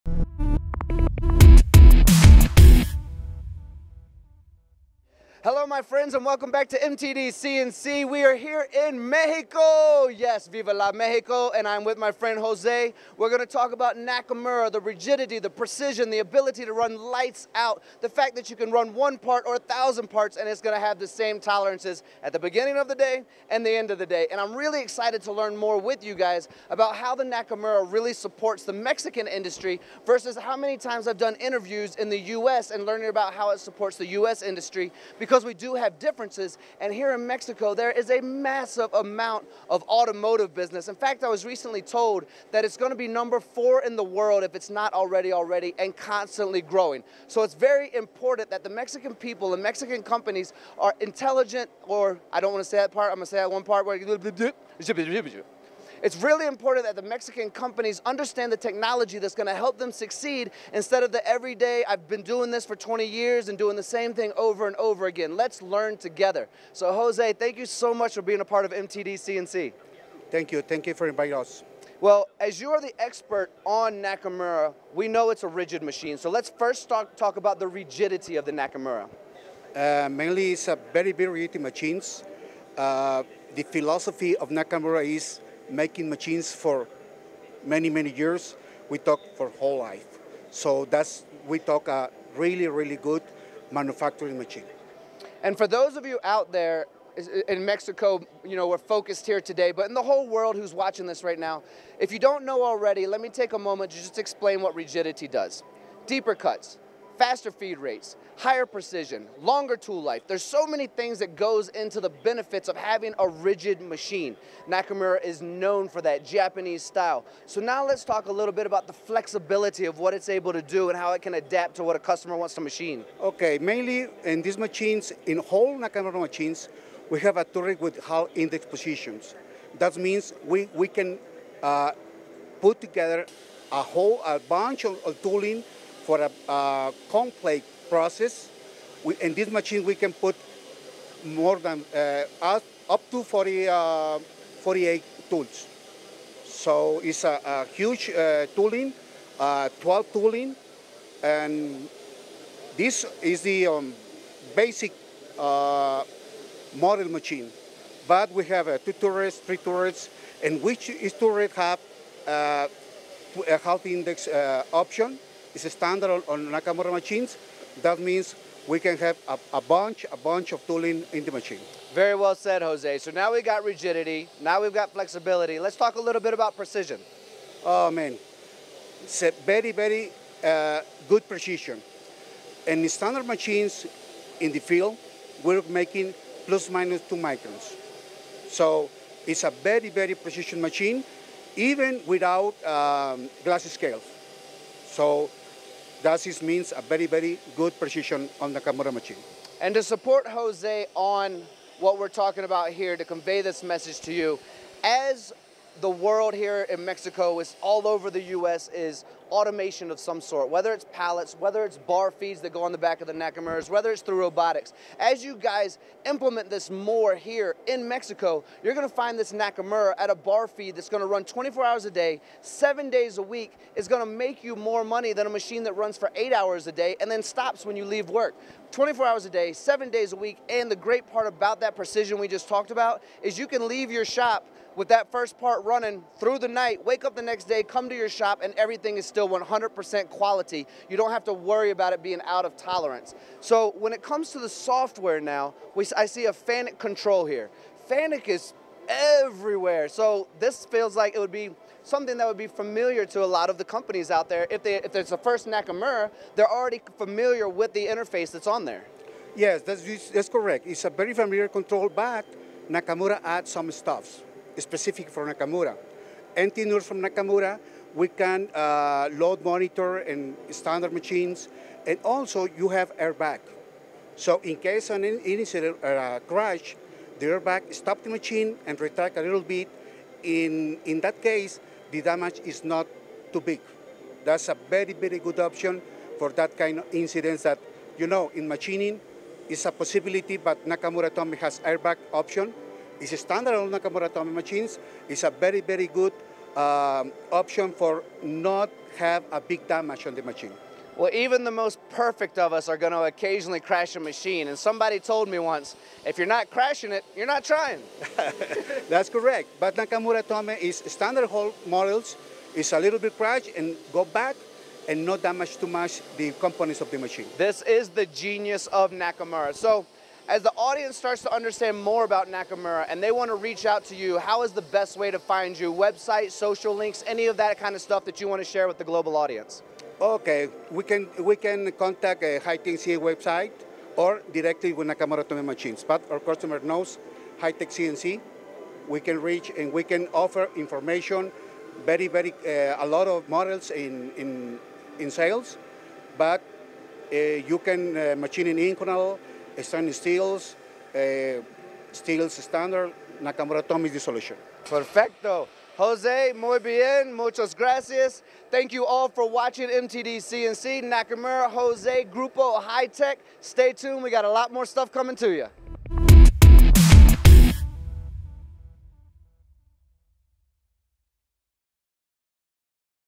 So why is Nakamura so popular in the automotive industry
Yoci talks about the rigidity of the Nakamura machine and why it works so well in the automotive industry. The rigidity helps with surface finish, precision tolerance, better tool life and more. Automotive manufacturing is so demanding and the Nakamura machine is perfect for delivering the right parts, every time.